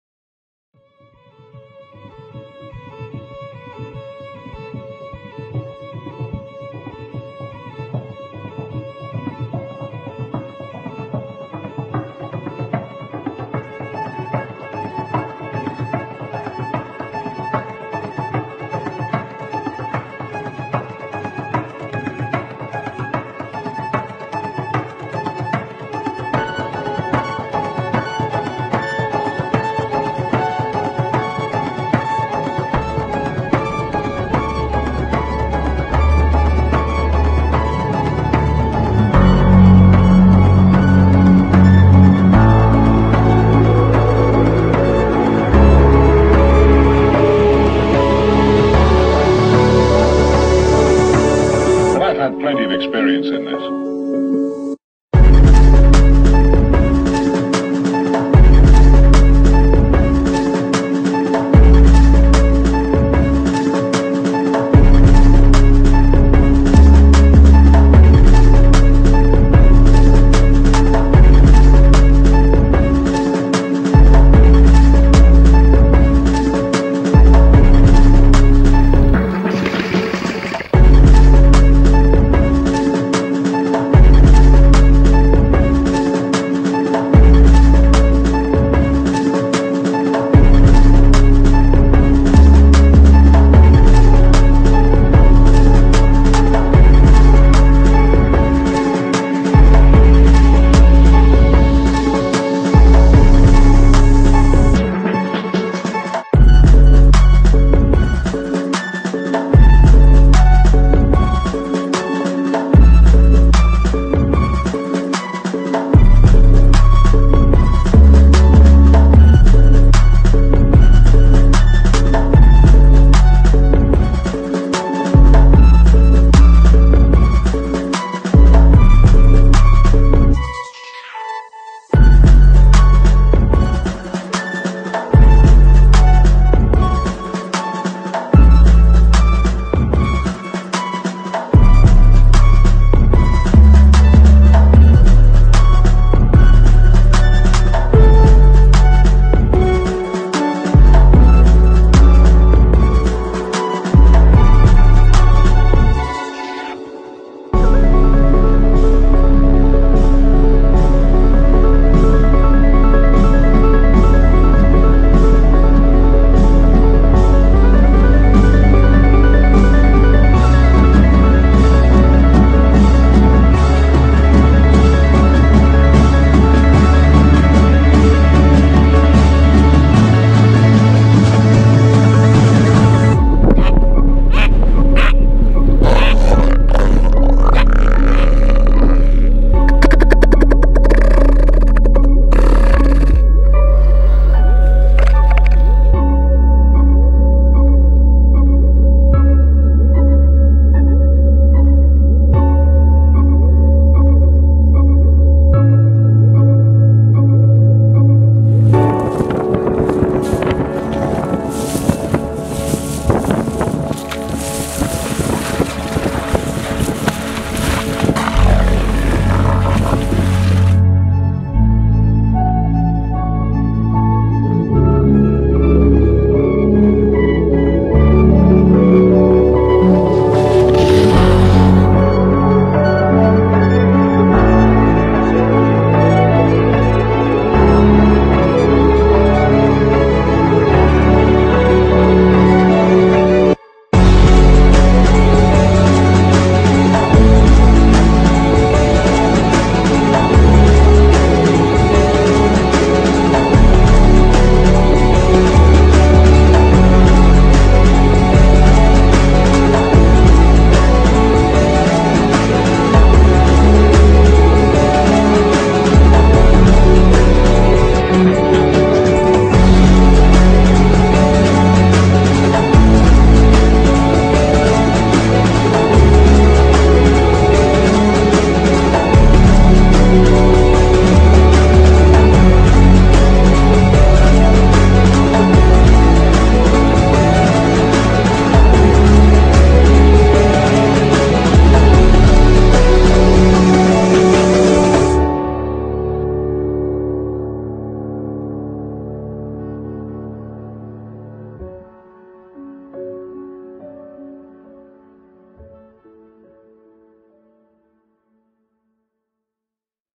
Winter in South Georgia, Antarctica
The Winter season down South. Skiing, ice-breaking in a jet boat, warring bull Elephant seals, Krill, South Georgia Pintails, Wandering Albatross chick and a struggling Leopard Seal... Lots of time-lapse, including the Milky Way.